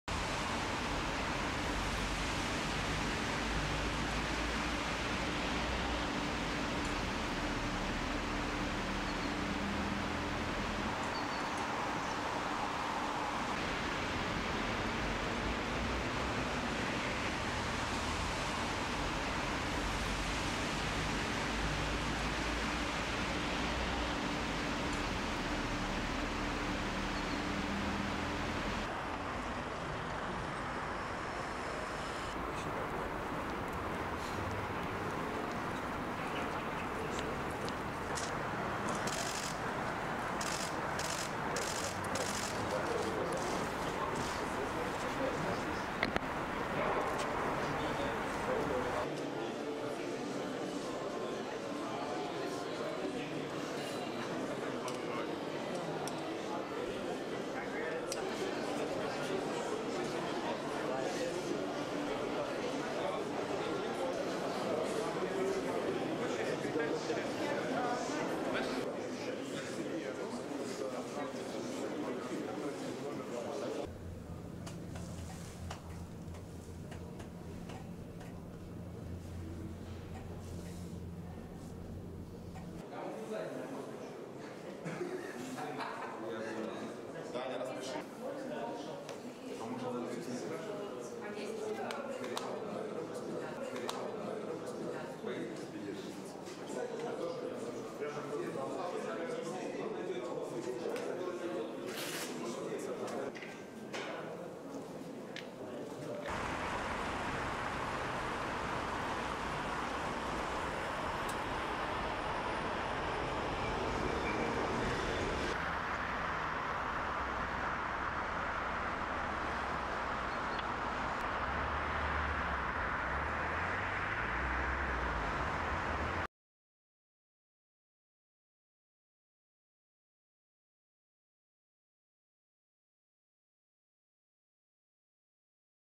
Wagner logos removed from group's former HQ in Saint Petersburg
The “Wagner Group” signs and all “Wagner” insignia were removed from the “Wagner Centre” business block in St. Petersburg on Saturday (July 1).

The building was opened in November 2022 and was seen as a major step by Wagner Group founder Yevgeny Prigozhin to publicise his military credentials and take a more public role in shaping Russia's defence policy.

The building was raided by the police on June 24 amid the mutiny.

Under the deal that ended the mutiny, fighters of the Wagner group were allowed to join their leader Yevgeny Prigozhin in exile in Belarus, to be integrated into Russia's regular armed forces or to return to their families.